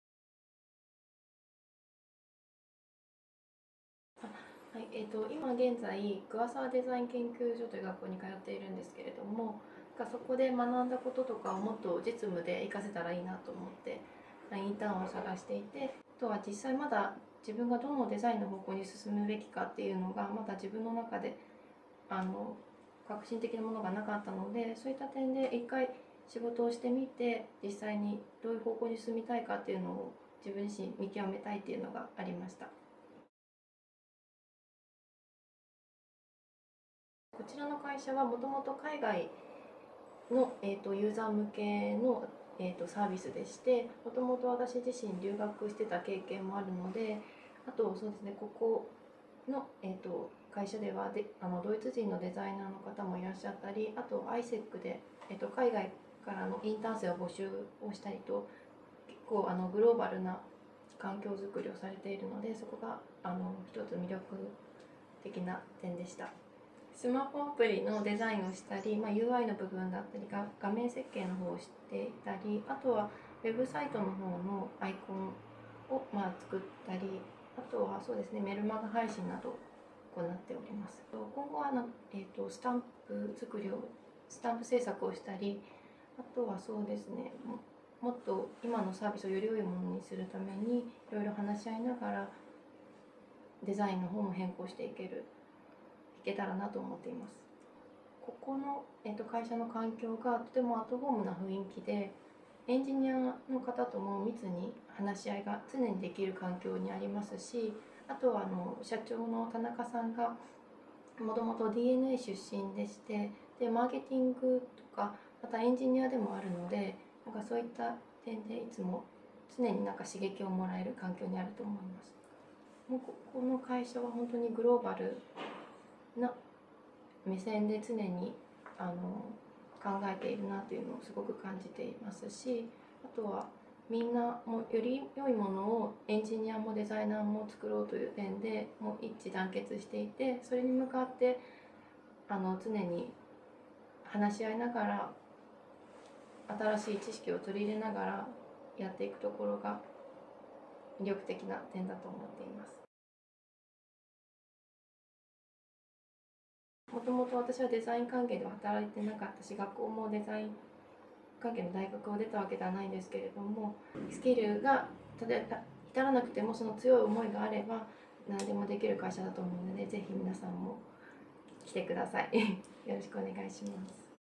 【株式会社Euno】専門学校生必見！デザイナーとして働くインターン生にインタビュー！ JEEK
「デザイナーとして働く学生の本音を知ろう！」
専門学校生、デザイナー志望者必見

インターン生　金田 恵美さん
2013年10月　デザイナーとしてインターンを開始

目次
1.インターンを始めたキッカケは！？
2.なぜEunoを選ばれたのですか！？
3.任されている業務は！？
4.Eunoの社内環境は！？
5.Eunoでのインターンの魅力は！？
6.最後に学生へメッセージをお願いします！